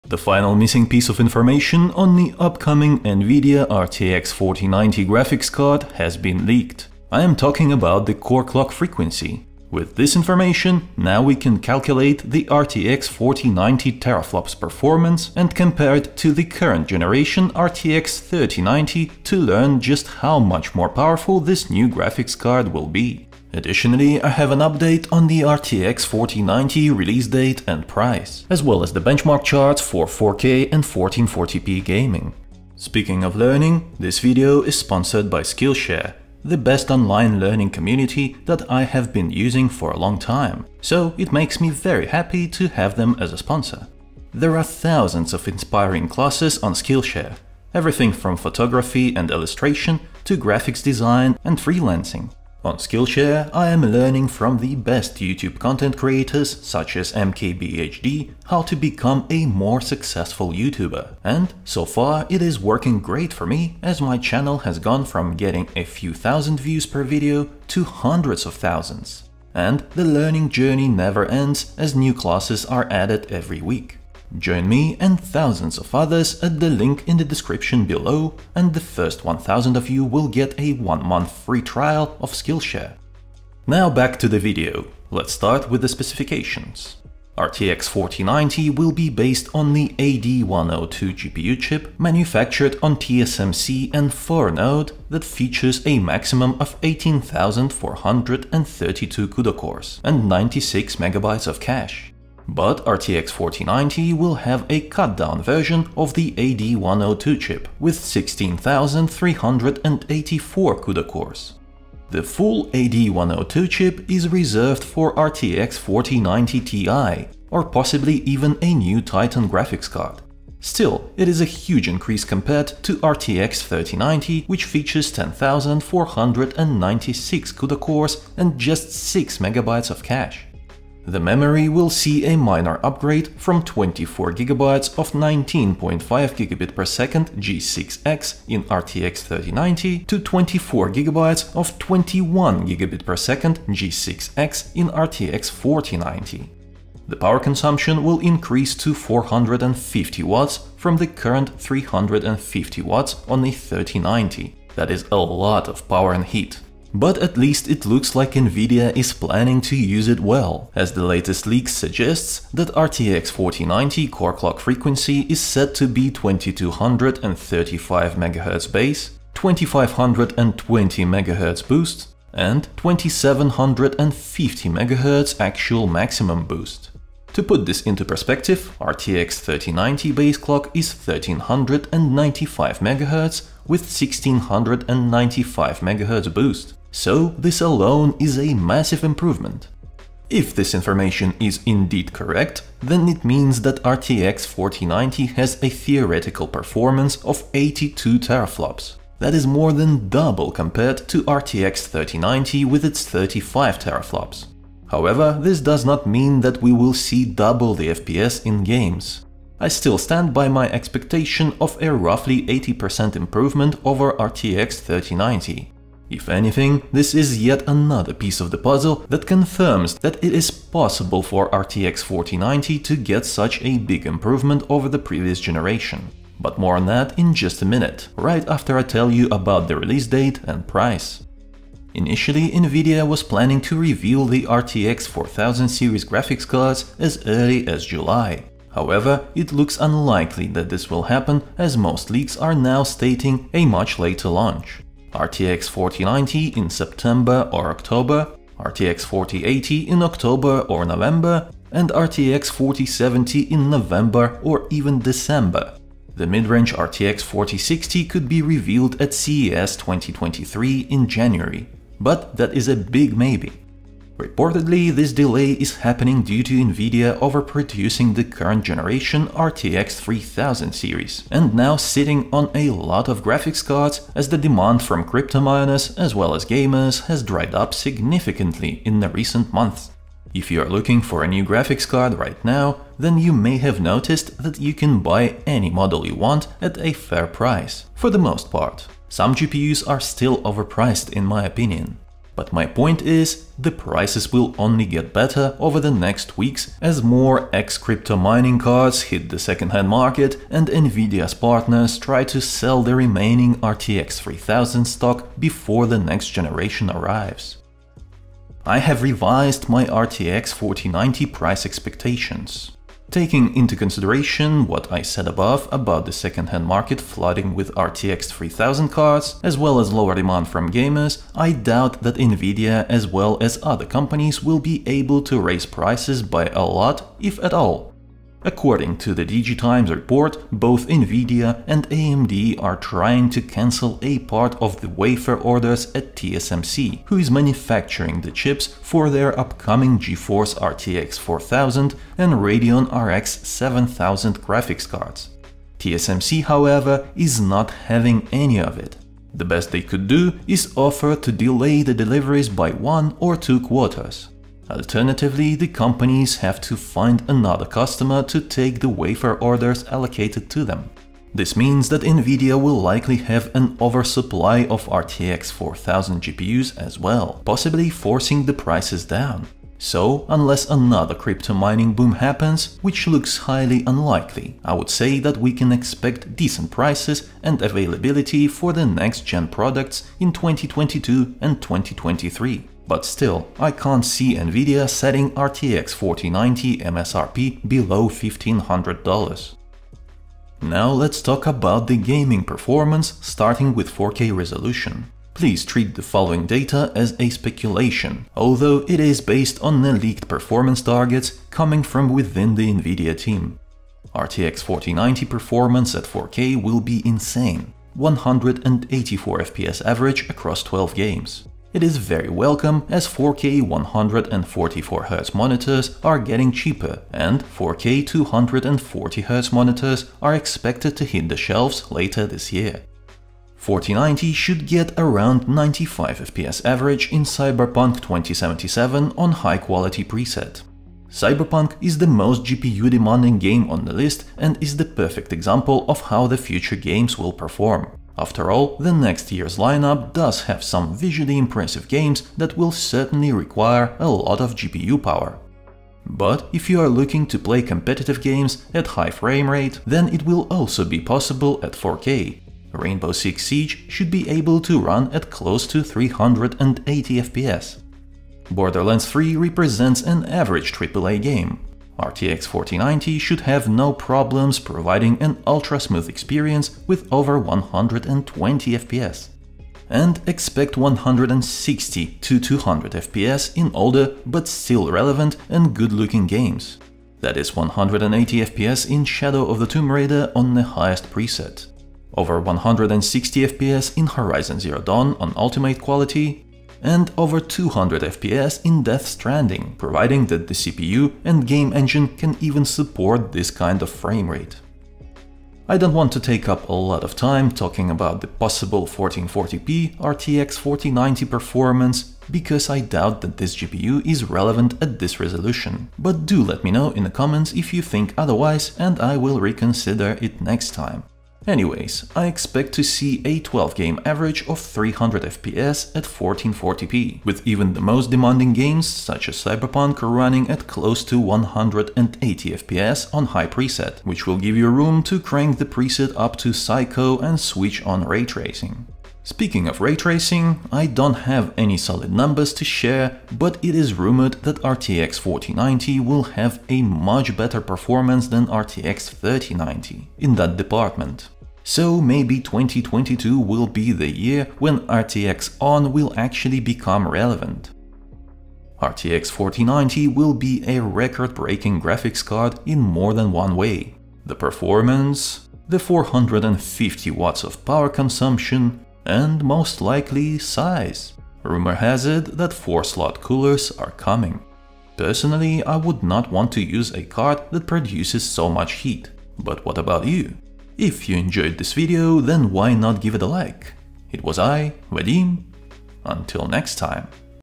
NVIDIA RTX 4090 is Better Than Expected [Everything Updated]
In this video, I tell you everything about the NVIDIA GeForce RTX 4090 specification, performance, release date as well as price. Additionally, I created benchmark charts comparing RTX 4090 vs RTX 3090 in gaming.

Correction: AMD did not cut its 5nm orders for Radeon RX 7000. They want to bring the fight to NVIDIA and grab market share. Which means that NVIDIA won't be able to delay RTX 4000-series launch by a lot. Next-gen high-end GPUs from both companies must launch in Q4 2022.

Timestamps:
0:00 - Intro
0:33 - Skillshare Ad
1:24 - Specs
3:52 - Release Date
5:08 - Price
6:30 - Performance 4K
8:31 - Performance 1440p
9:08 - Final Thoughts